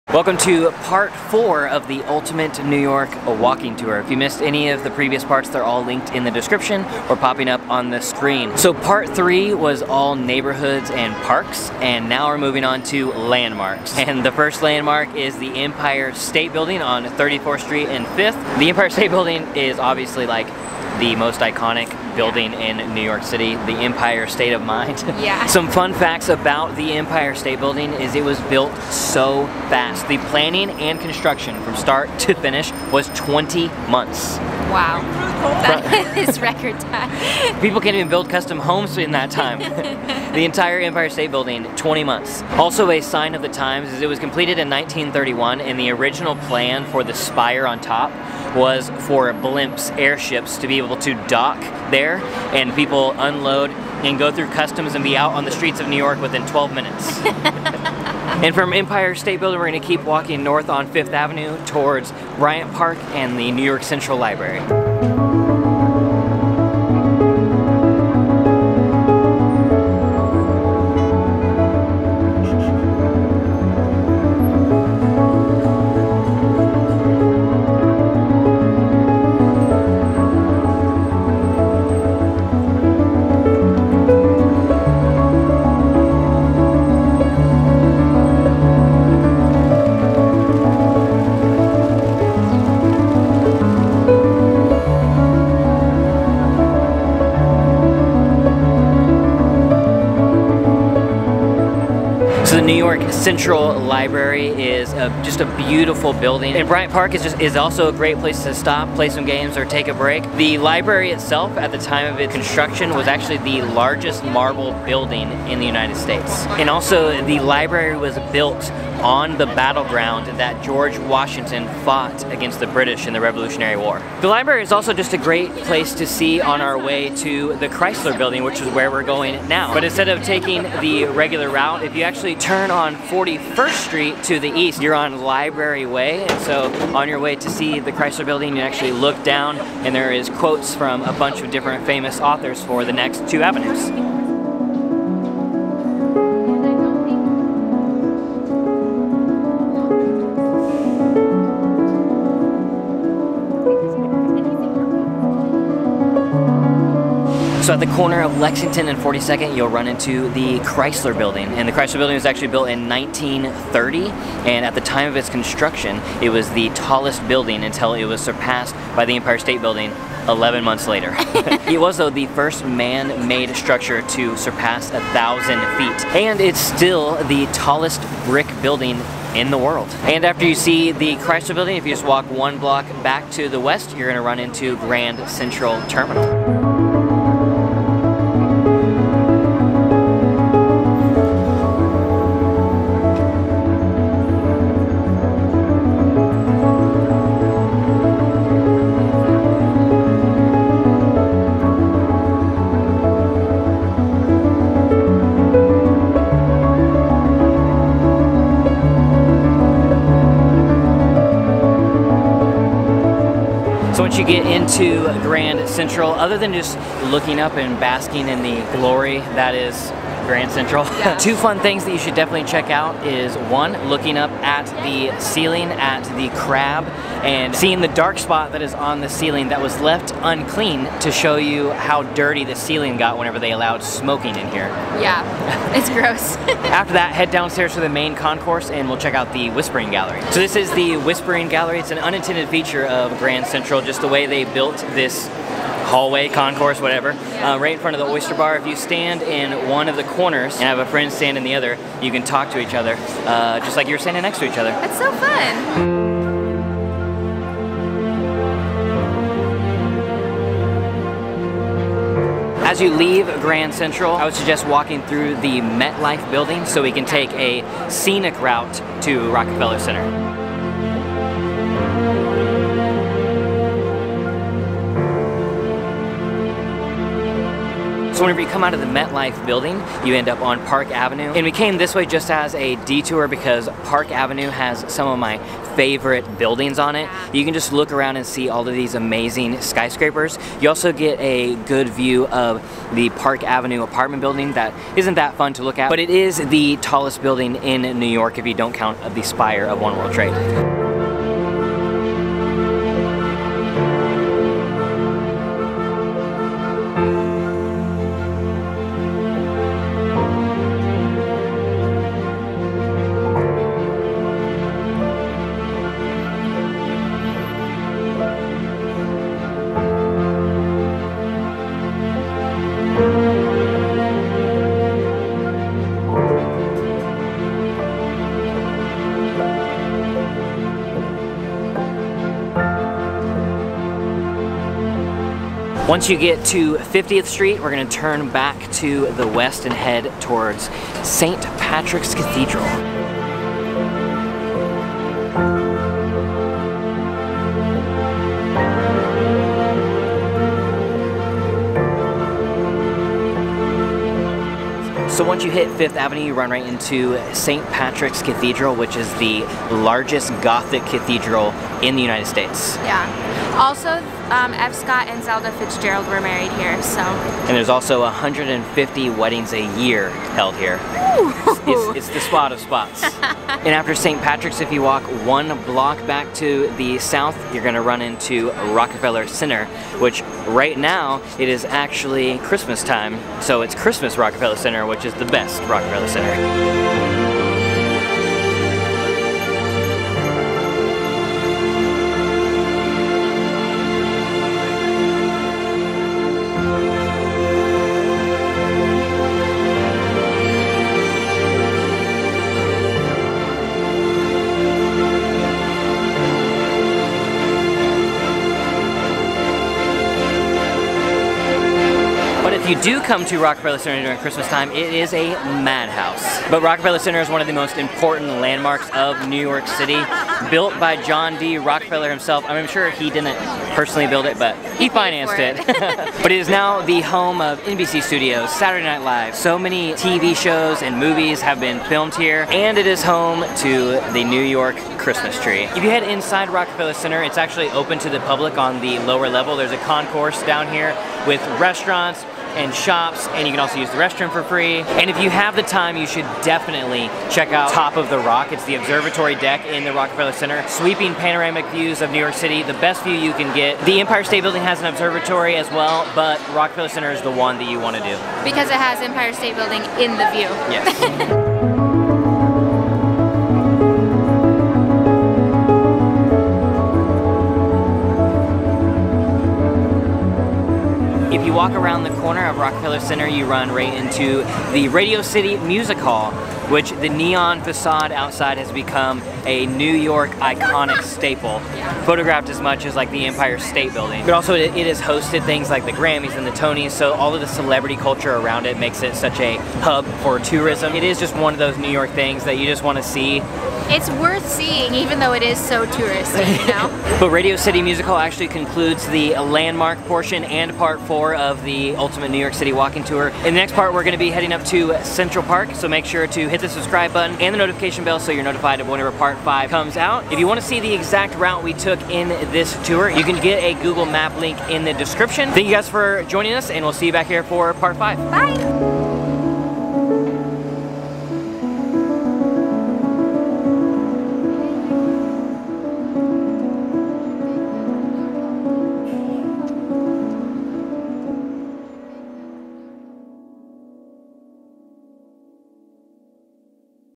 Ultimate NYC Walking Tour - Part 4
See NYC in one day (or break it up in 2 or 3 days). This is part four of a five part series, walking NYC from the southern tip of Manhattan to Central Park!

My Gear:
Vlog Camera
Lens I have
Lens I want
Bendy Tripod
My electric skateboard